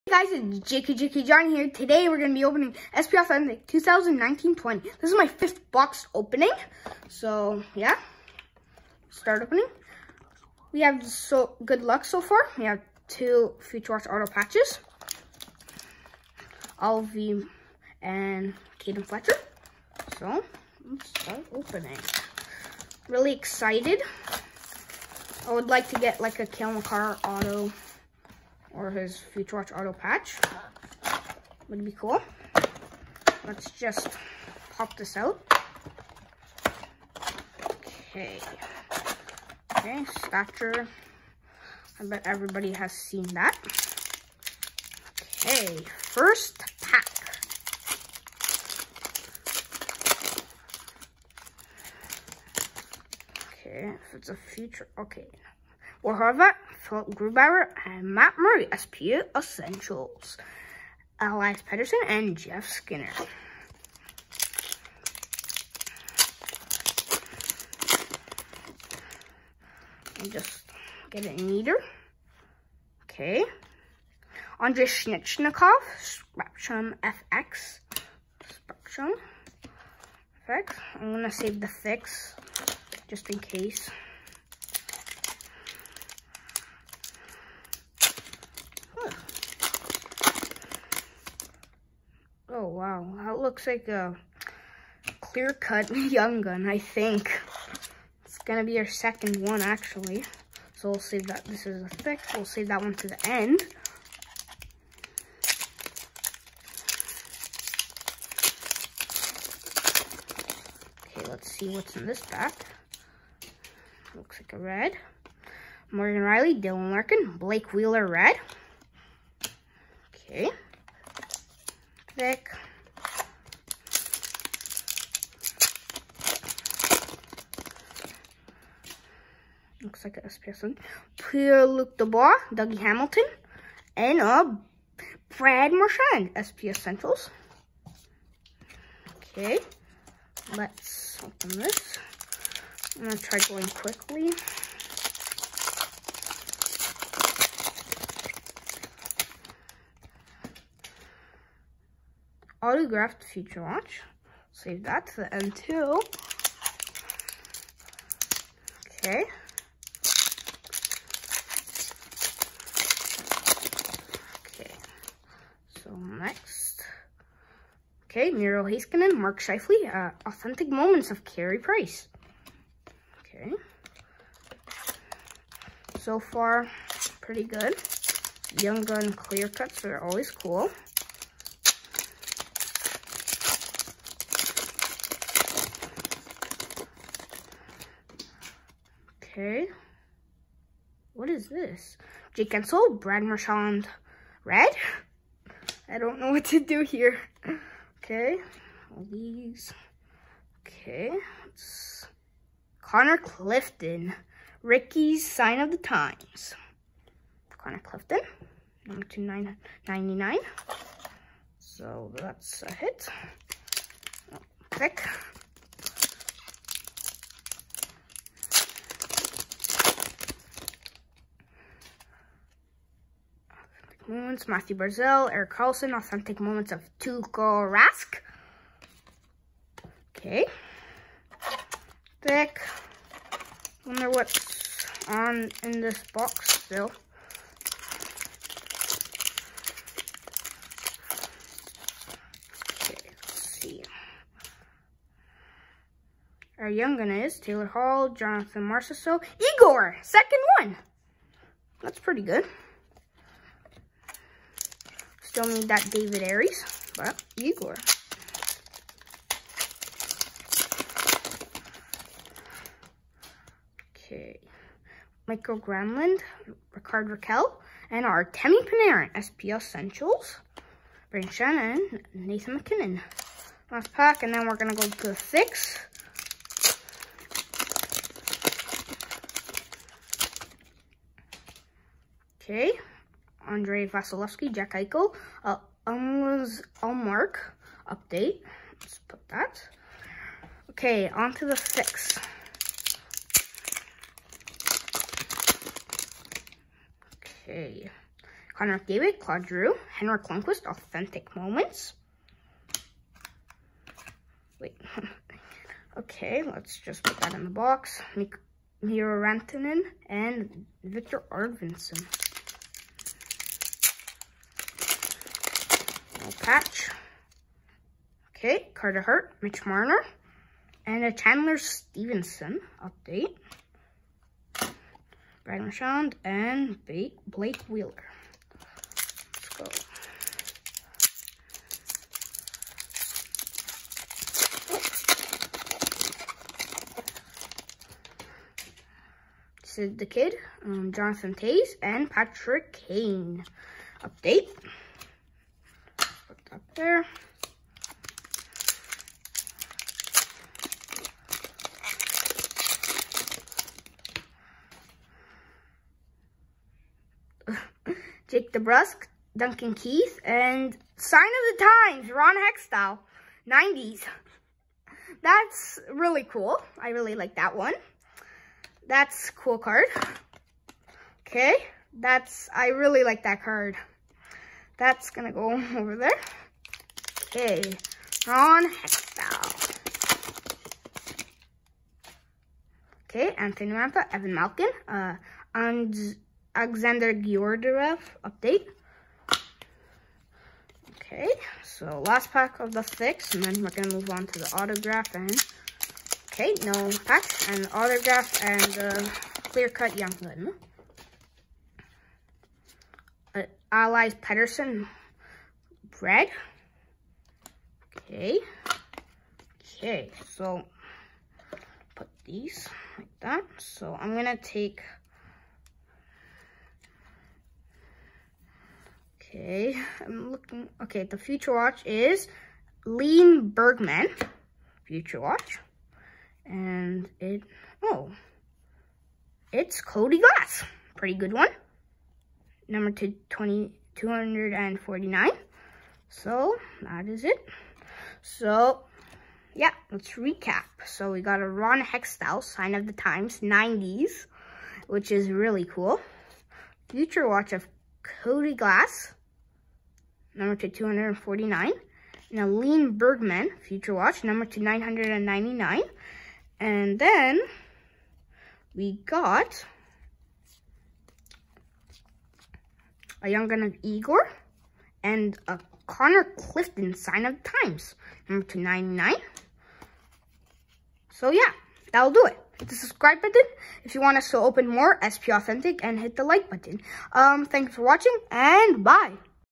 Upper Deck Hockey SP Authentic 2019-20 box opening - 3 autos, Young Gun, 2 Future Watch HITS - box 5
Opening a box of Upper Deck Hockey 2019-20 SP Authentic - our 5th box. Cool hits -- 3 autos, 2 future watches, one good Young Gun. Really like opening SP Authentic.